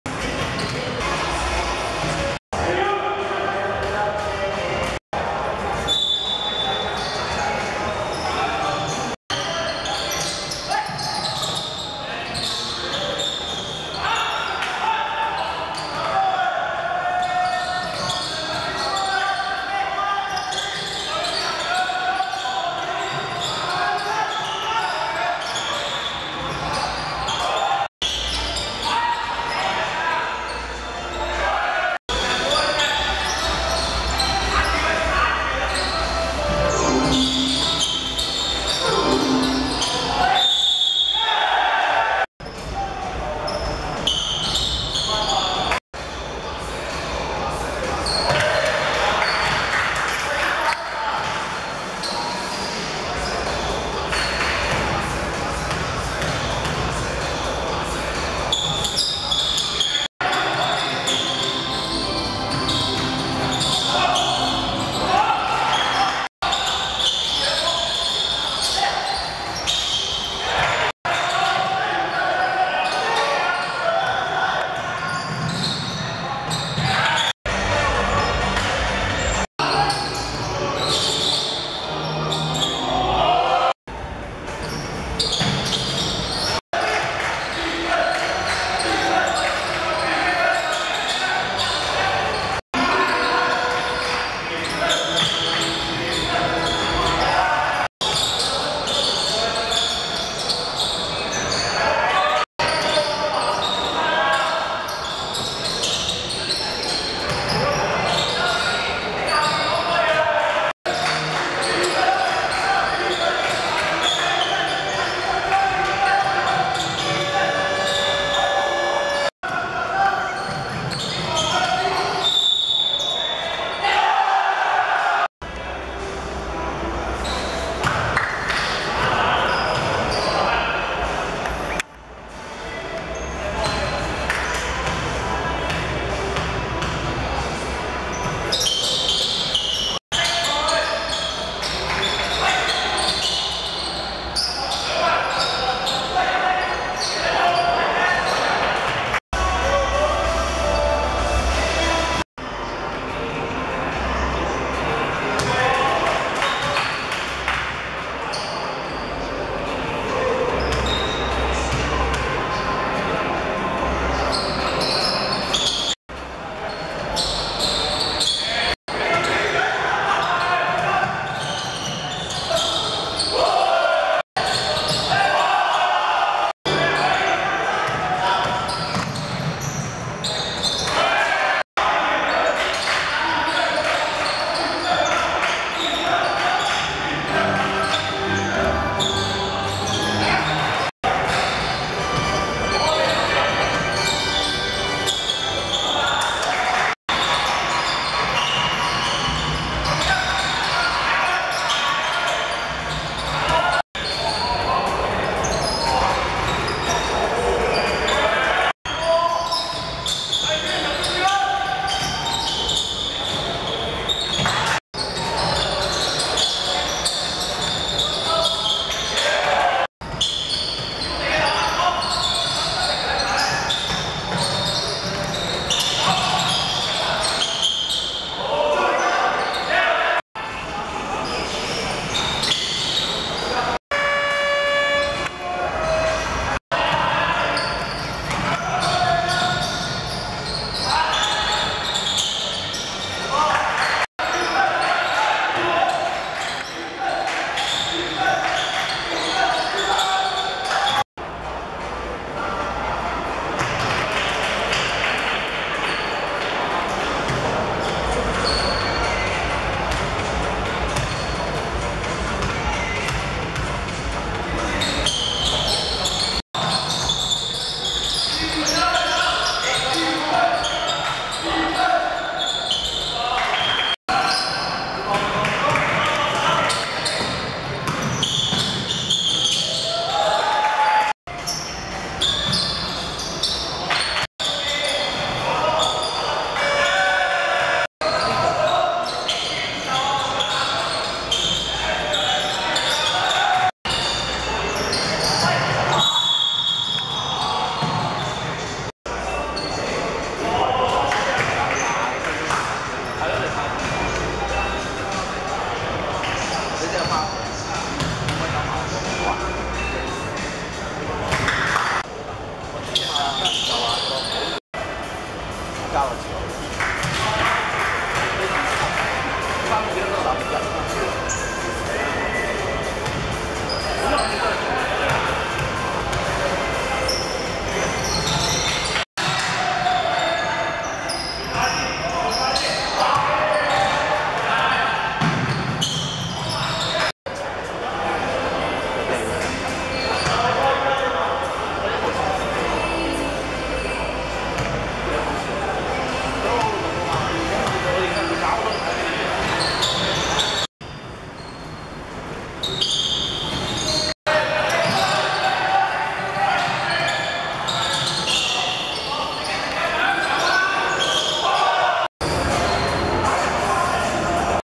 浸大vs港大(2015.8.26.Jordan Take Flight Invitational大專籃球賽)精華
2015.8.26.修頓塲館 20:30 浸大(白) 38 : 54 港大(黑)